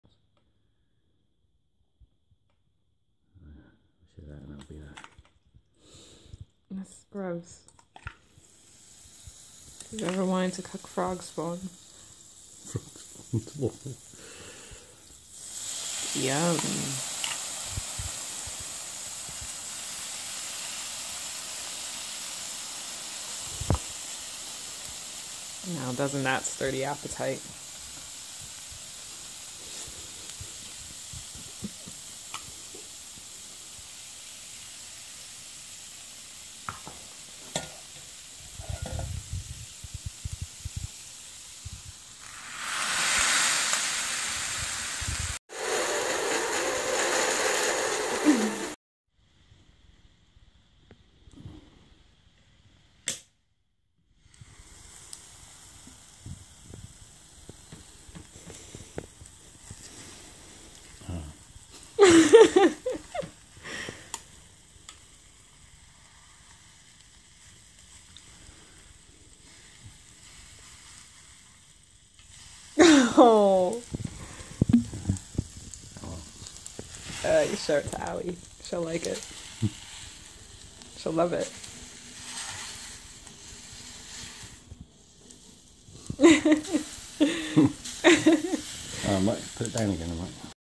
Frog spawn waffles! 🐸
Making waffles from frog spawn (tapioca pearls)